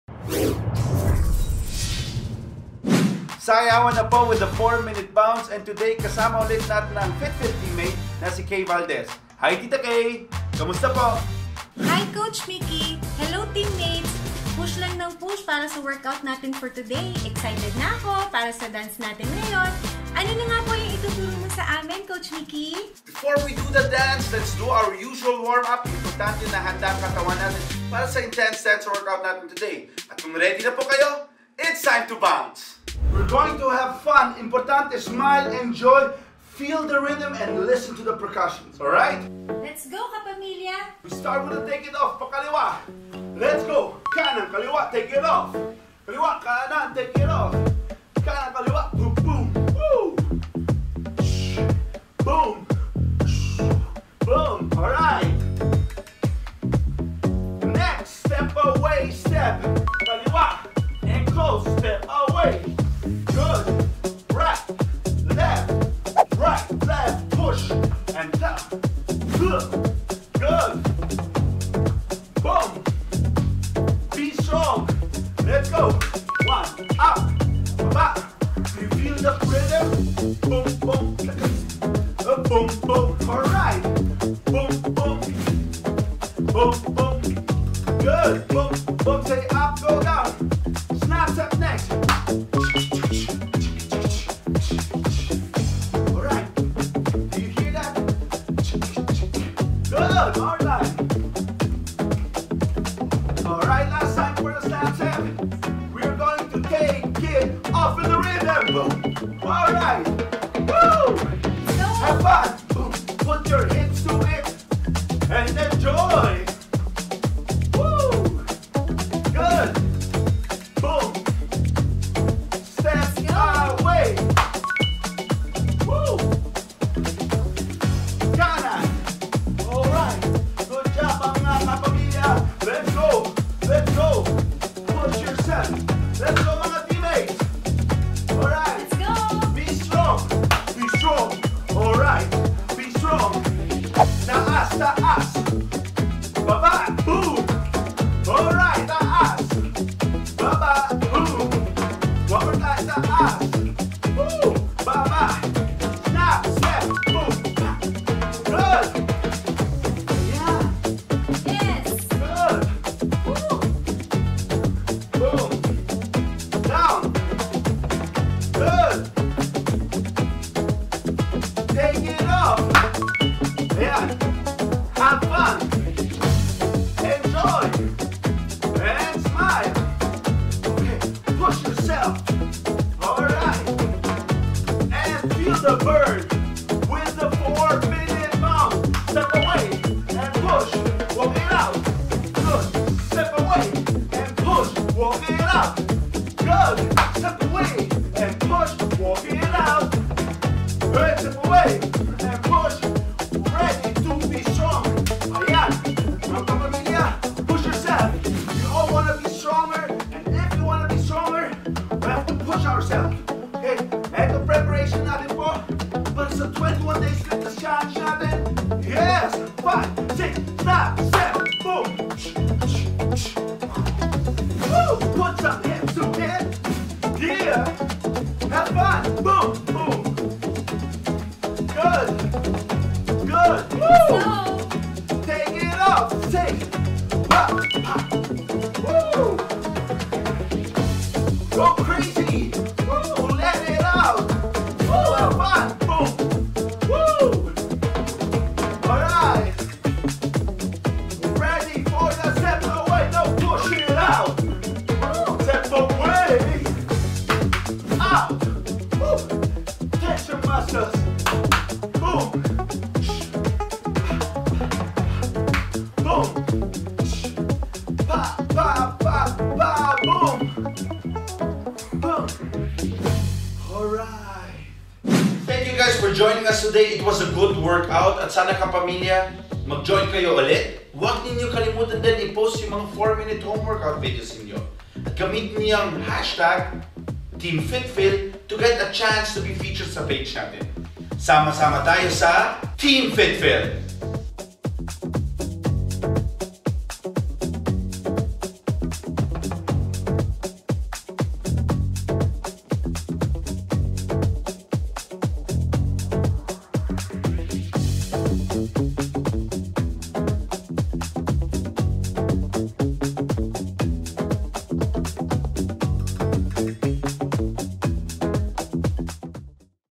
4-Minute Dance Workout with FitFil Teammate Kaye | Team FitFil Episode 32
Sayawan na, Team FitFil! Anyone can be part of our journey to a healthier life! Kaya naman sabayan na si Coach Mickey Perz at ang ating FitFil Teammate na si Kaye Valdez in this 4-minute dance workout!

Follow Team FitFil on social media!